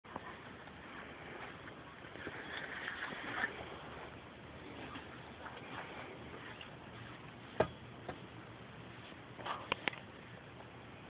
ribs and football
smoking ribs for the razorback game!